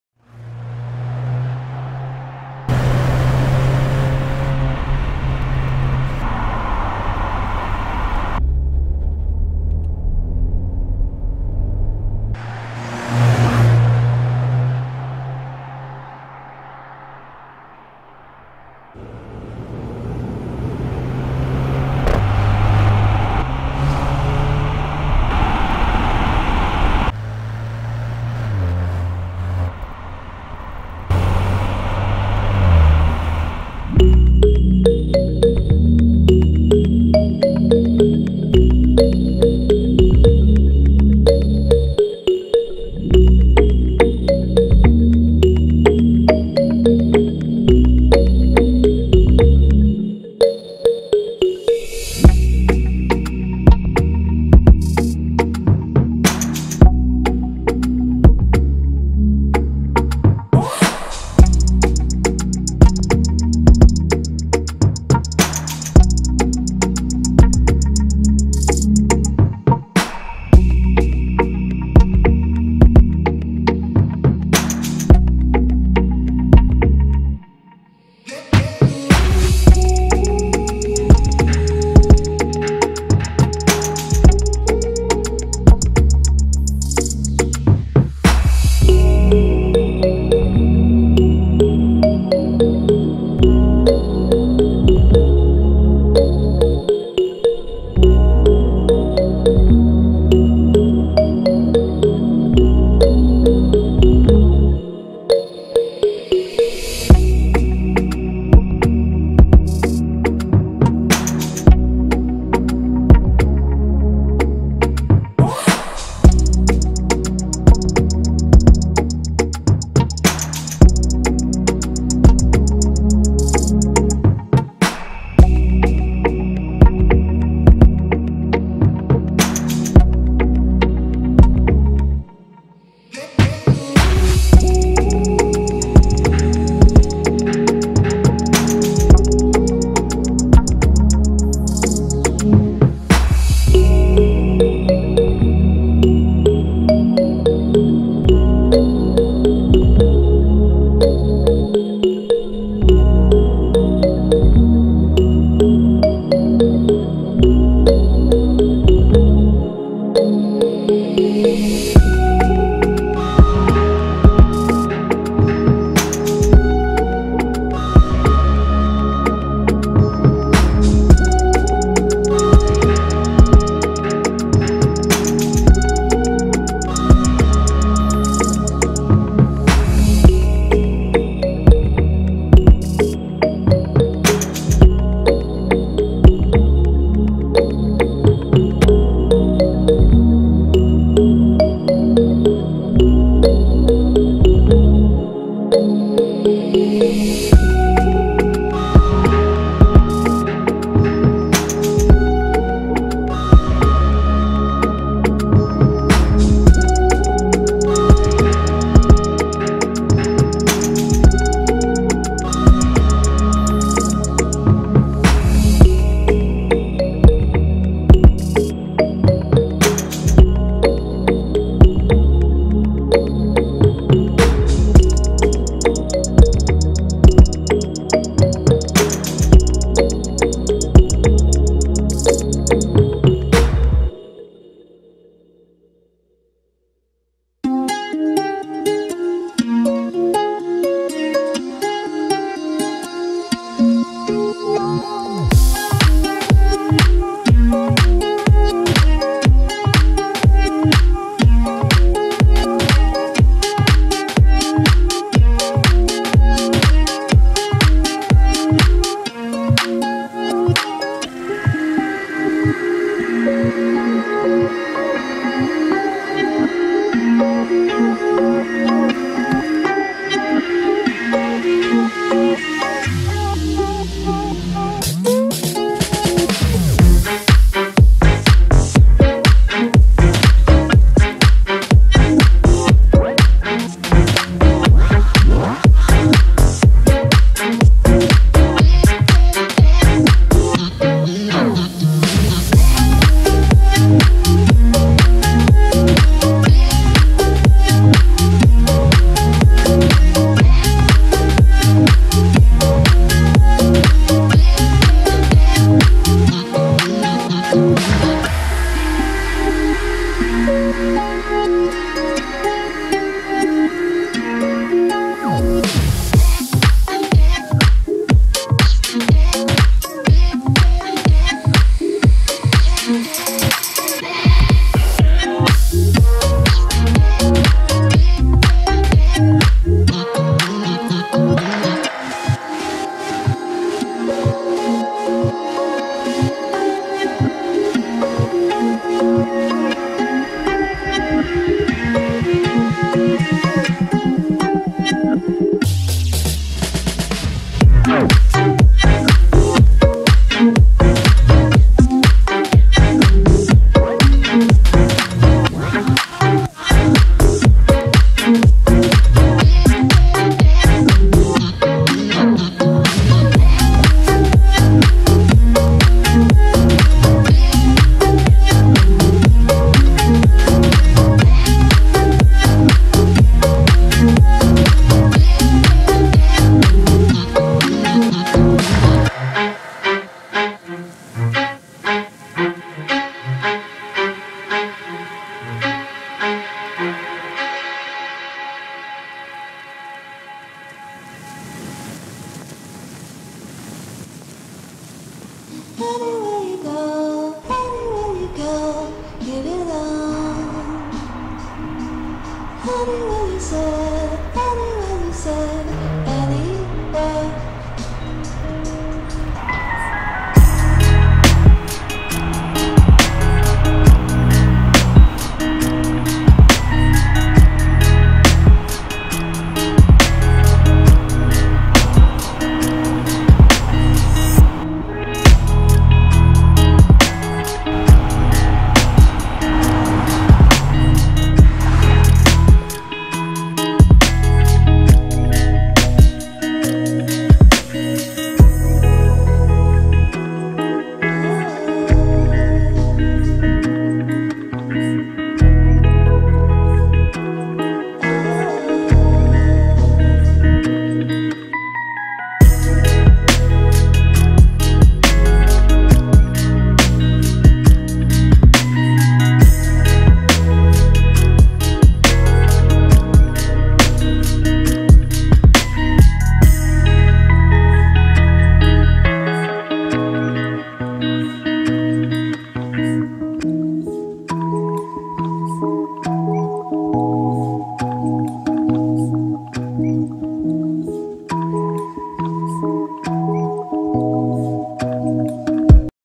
New 2023 Mercedes AMG C 43 - Elegant Sedan - Exterior Interior and drive
New 2022 Mercedes AMG GT 63 S E Performance - Jupiter red - Interior Exterior and Drive
00:33 - Exterior Mercedes AMG C 43 T-Model
04:30 - Interior Mercedes AMG C 43 T-Model
06:37 - Test Drive  Mercedes AMG C 43 T-Model